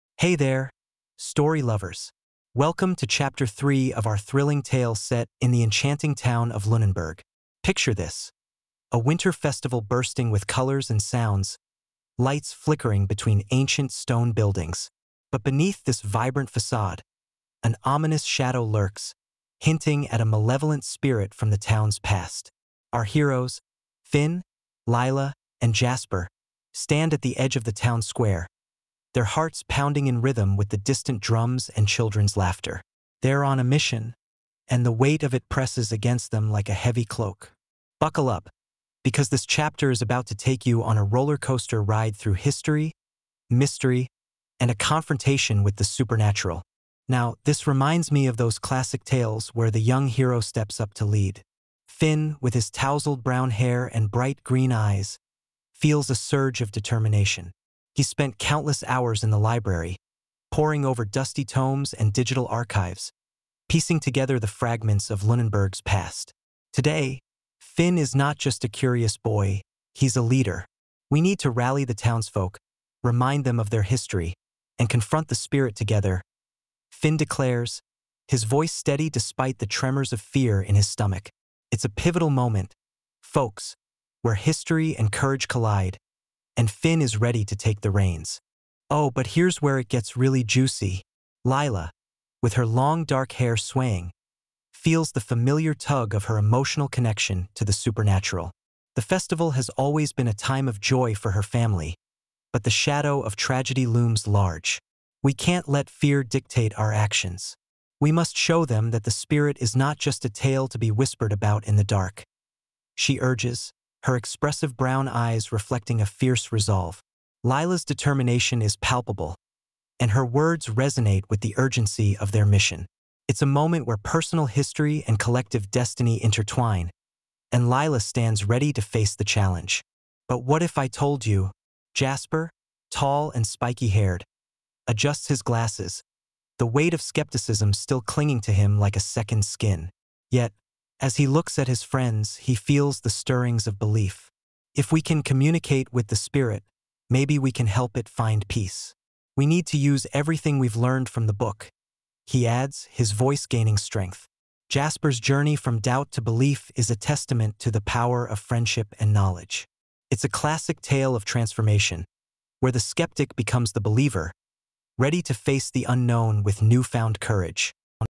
Winter Solstice Mystery | Ch 3 - Confronting Shadows (2024)
In the climactic scene, Finn, Lila, and Jasper must rally the townsfolk to face the malevolent spirit. This chapter is a test of courage, unity, and legacy.

The Winter Solstice Festival is about to begin, and the trio knows it's their final chance to face the malevolent spirit before it's too late. Finn's leadership, Lila's resolve, and Jasper's transformation lead to a pivotal confrontation with the Mayor, who must remind the townsfolk of their rich history and face the spirit together. The result is not just a victory over fear but a celebration of unity and courage.

Timestamps:
00:00 - Intro & Hook
00:45 - Leadership Calls
1:00 - Rallying the Townfolk
1:30 - Mayor's Speech
02:00 - Confrontation
02:30 - Celebration of Unity